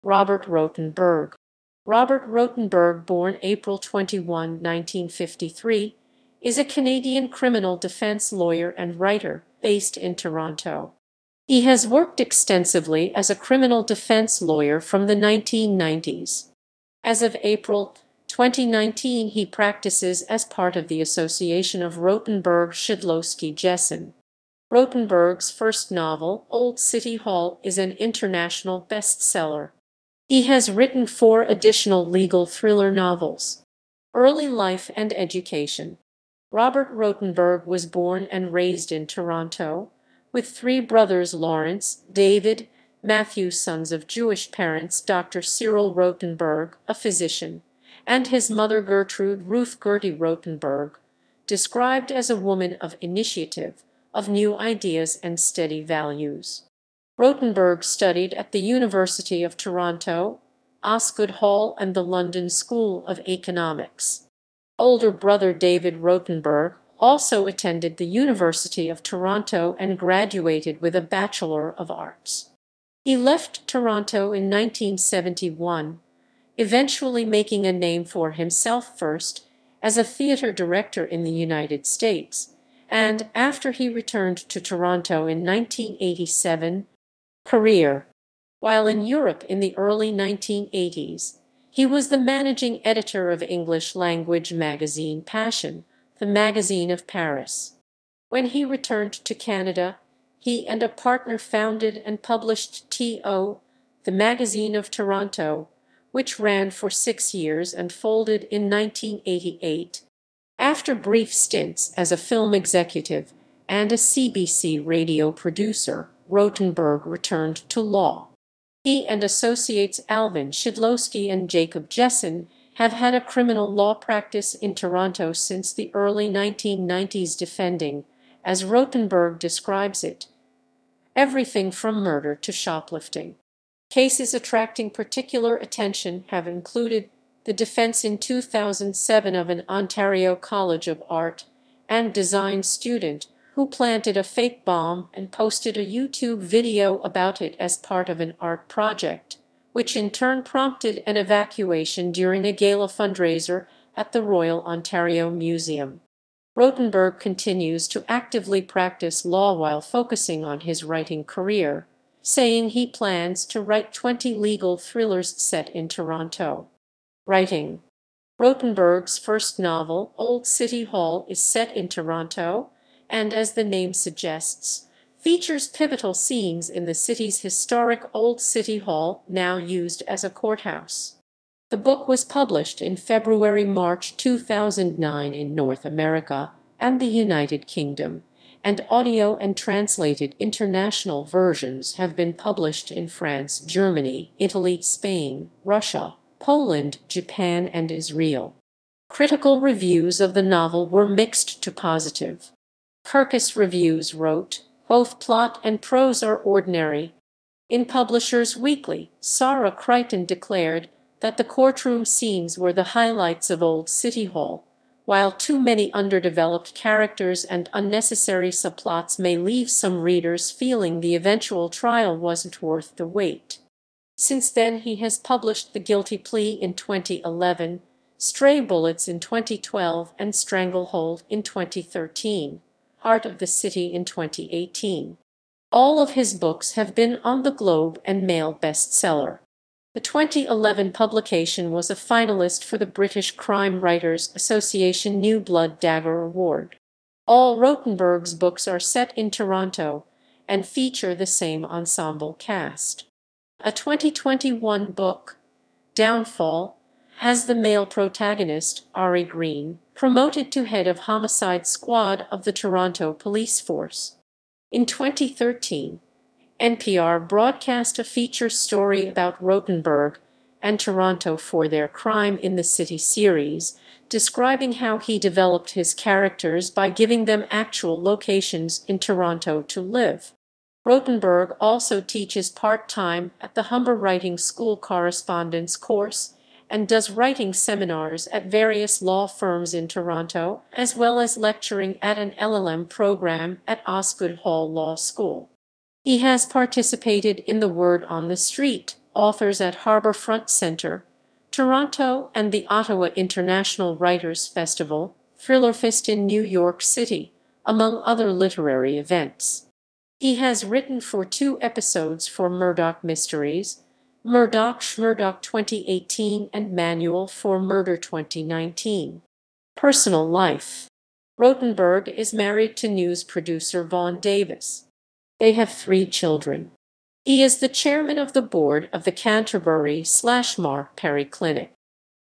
Robert Rotenberg
Robert Rotenberg (born April 21, 1953) is a Canadian criminal defence lawyer and writer, based in Toronto. He has worked extensively as a criminal defence lawyer from the 1990s. As of April, 2019 he practices as part of the association of Rotenberg Shidlowski Jesin. Rotenberg's first novel, Old City Hall is an international best-seller. He has written four additional legal thriller novels.